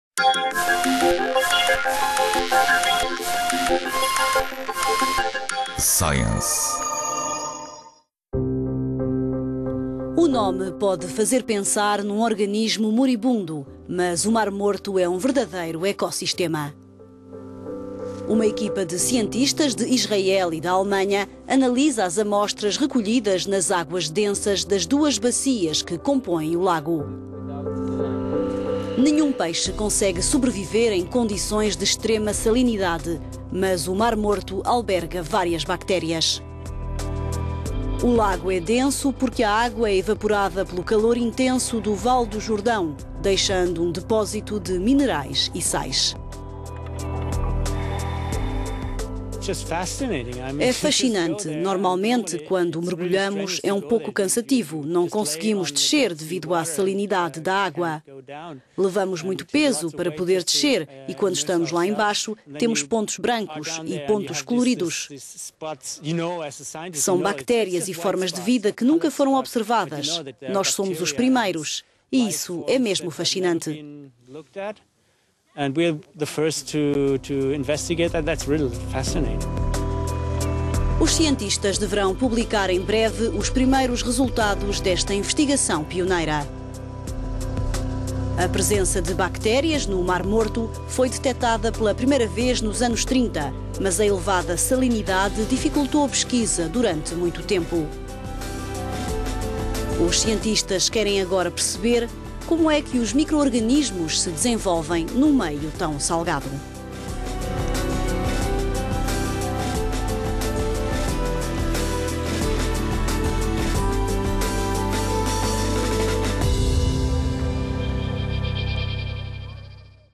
euronews science - Mar morto não está assim tão morto
O nome pode fazer pensar num organismo moribundo mas o Mar Morto é um verdadeiro ecossistema. Uma equipa de cientistas de Israel e da Alemanha analisou amostras de bactérias recolhidas nas águas densas das duas bacias que compõem o lago.